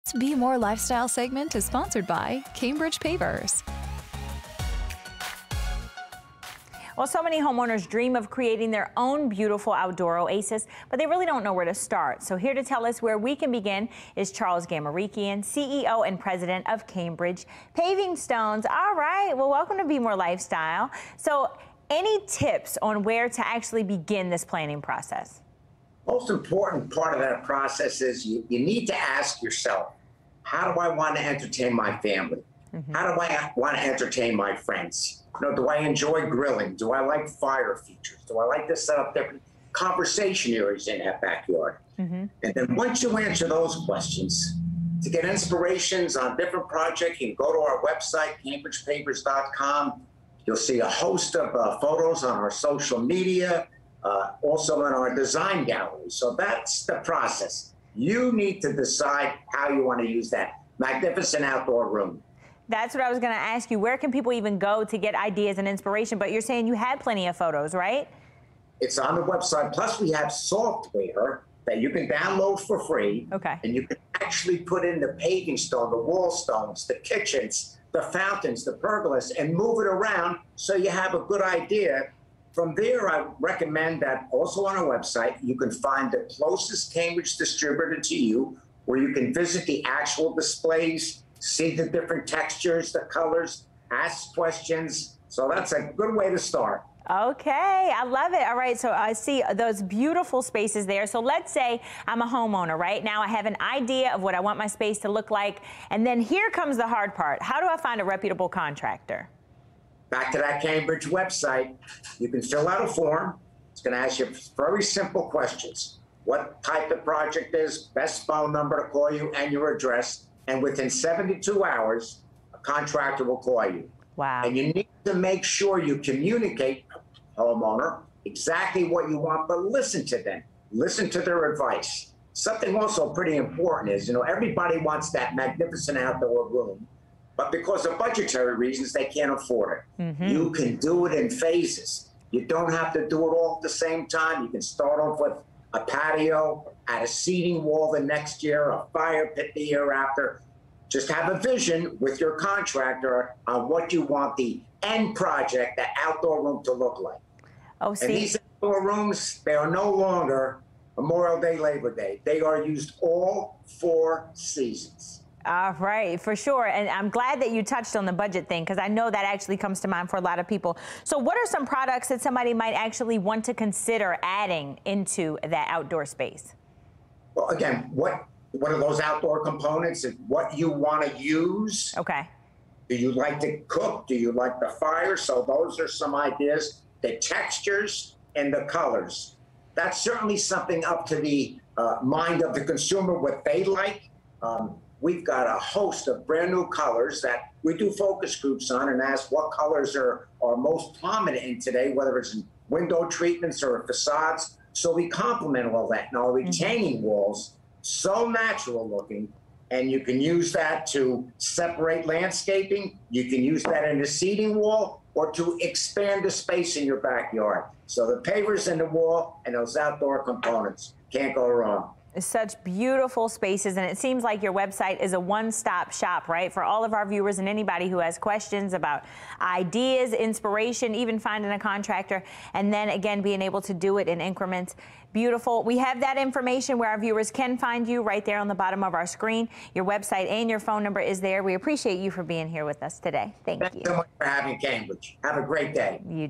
Cambridge Pavers: Summer Outdoor Living
Many homeowners dream of creating their own beautiful outdoor oasis but they don't know where to start.  Charles Gamarekian is the CEO and President of Cambridge Pavers.  He shares how you can begin the process.